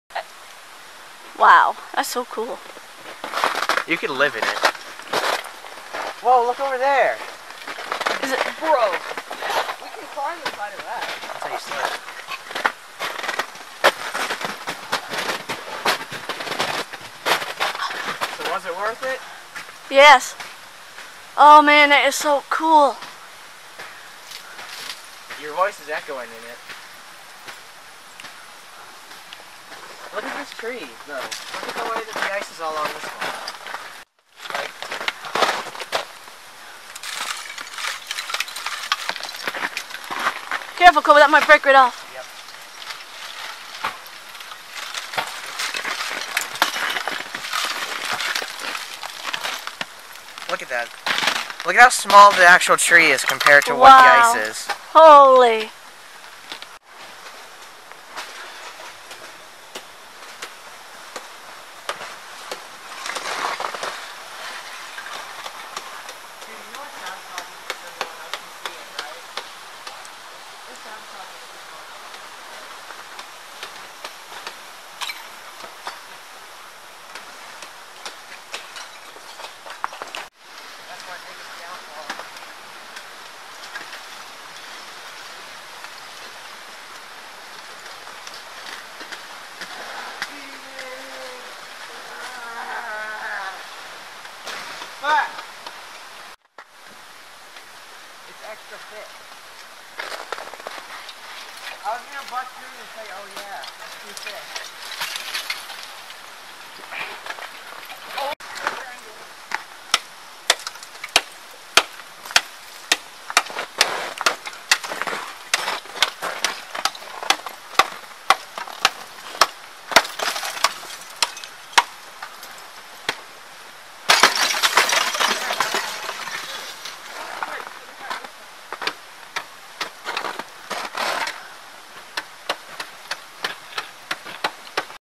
thick ice forms on trees from spray of waterfall
waterfalls spray water on trees in winter and it freezes in some cool formations.